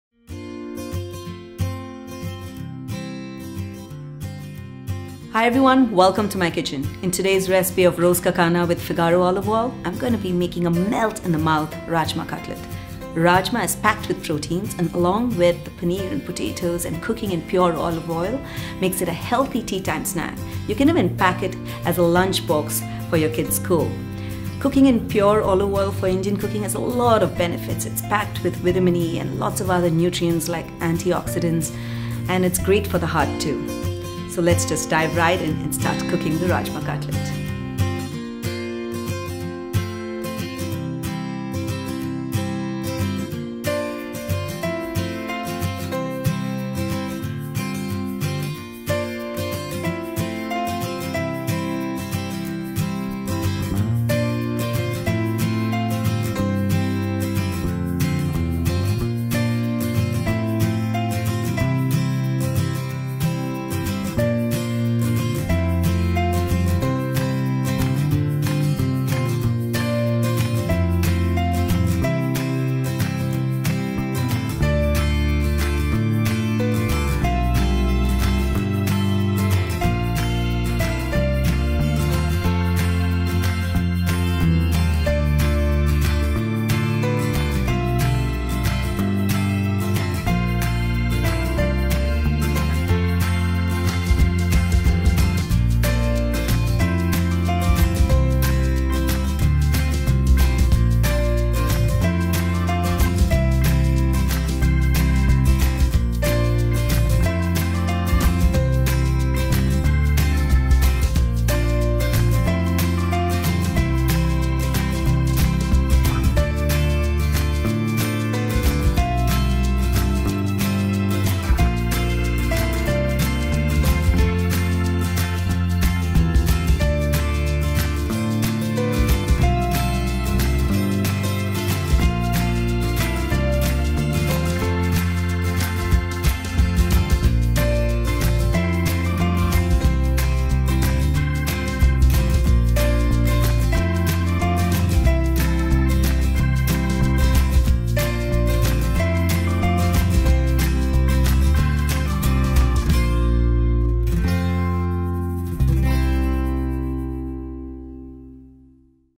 Rajma Malai Cutlet - Roz Ka Khana with Fiagro Olive Oil
This Holiday Season, make this melt in the mouth Rajma Malai Cutlet that's packed with protein from paneer. It makes a great appetizer for parties. Serve it along with a yogurt dip or mint chutney to make a lip smacking dish. This the first recipe from our series of RozKaKhana with FigaroOliveOil